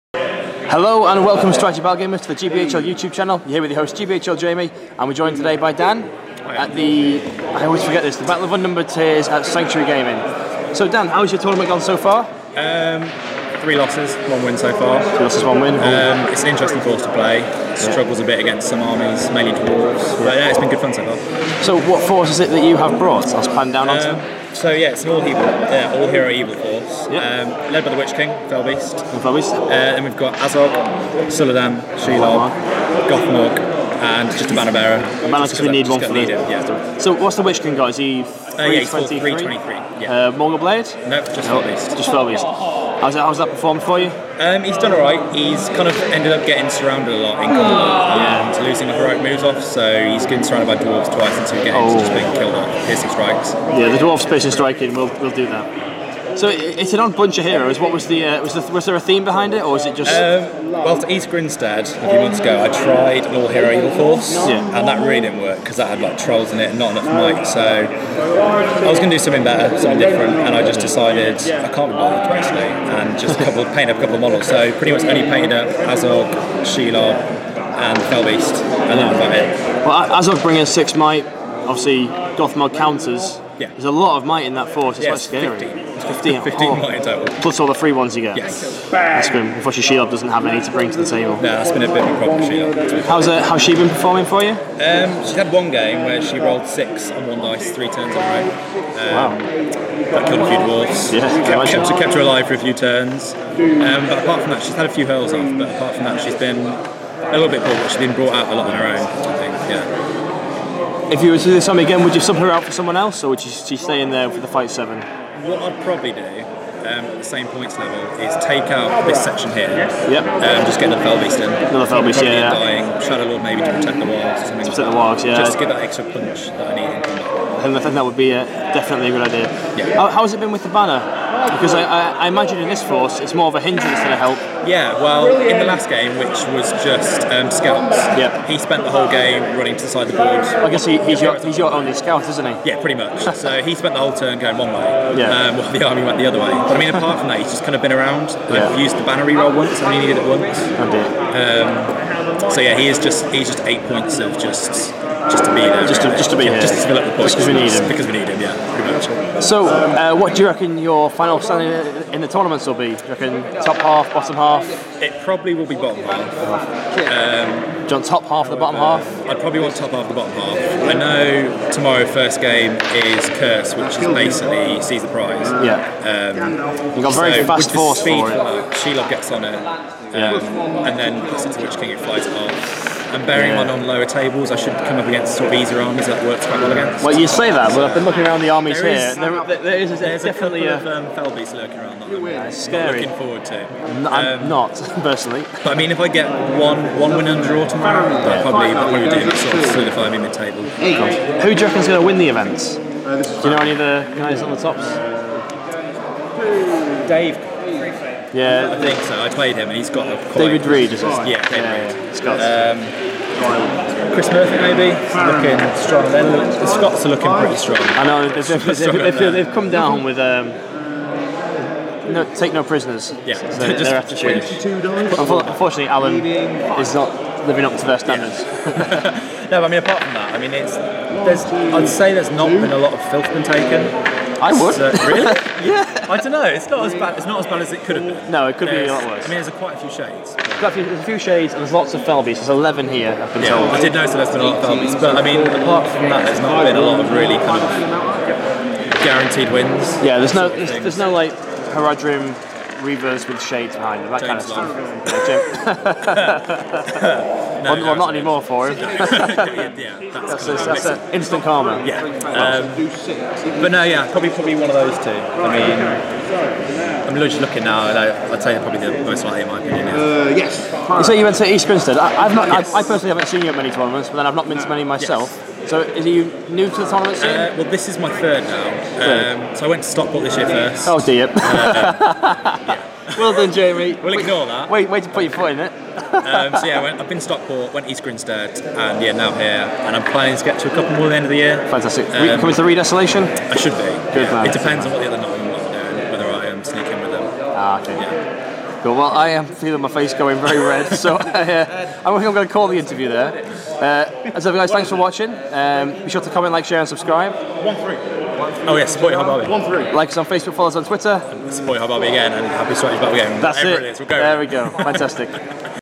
Player Interview - All Evil Heroes - Dan Huckvale
GBHL Jamie discusses Dan's Army choice for the Battle of Unnumbered Tears.